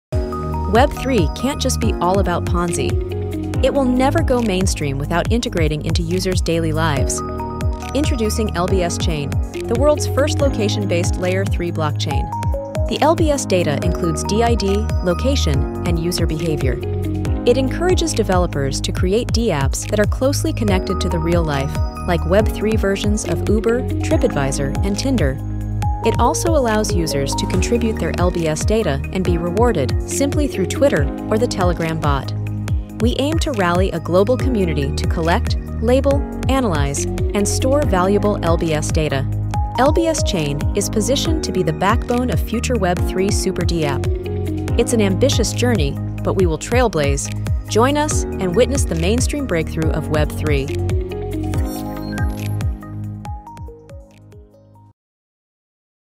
LBS Chain Official Trailer
Web3 can't just be all about Ponzi. It will never go mainstream without integrating into users' daily lives. Introducing LBS Chain, the world's first location-based Layer 3 blockchain.